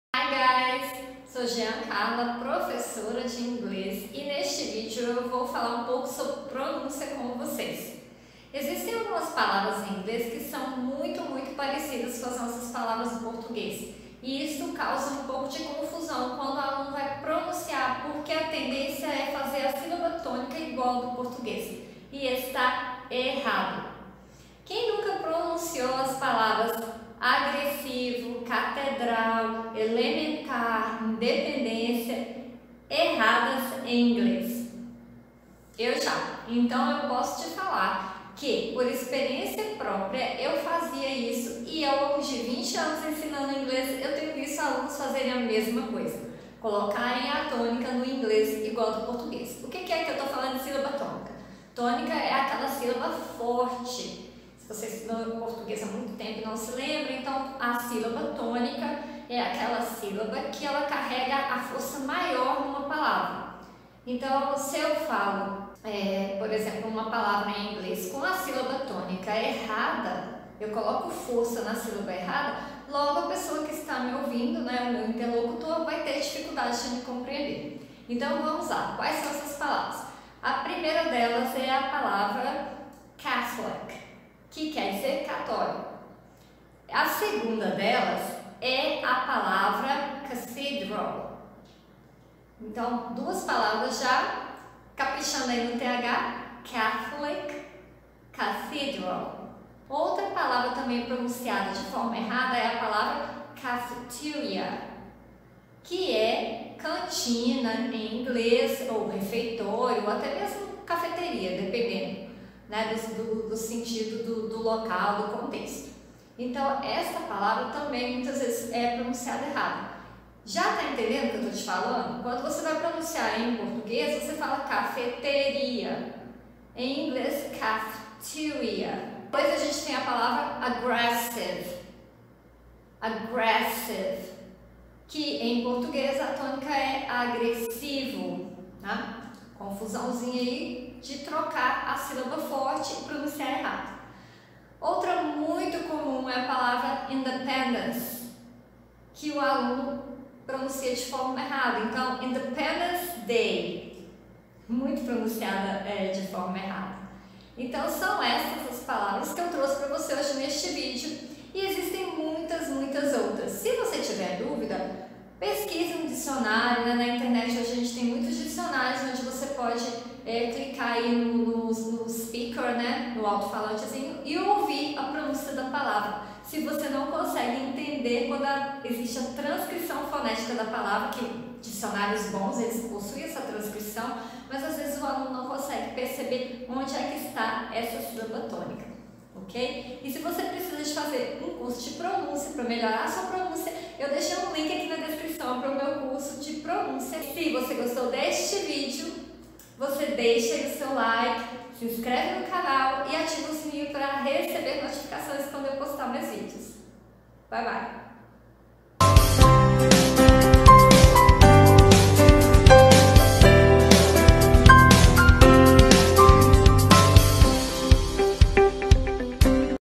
Como pronunciar cathedral, independence, catholic...
Neste vídeo te explico como pronunciar as palavras cathedral, agressive, independence, cafeteria em inglês.

GEAN CARLA PEREIRA - Mestre em Literaturas em inglês pela UFMG, professora de inglês e espanhol na Way Idiomas (GOVERNADOR VALADARES/MG).